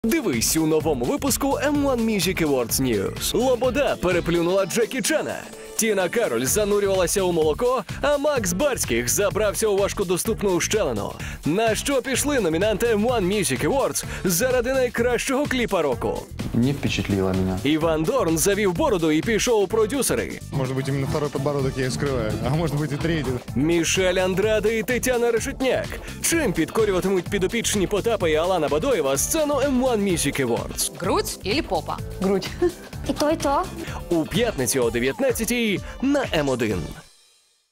M1 Music Awards News. Анонс - 02.12.2016
Друга Церемонія нагороження M1 Music Awards від телеканалу М1 та Найочікуваніший гала-концерту року відбудуться 10 грудня у київському Палаці Спорту.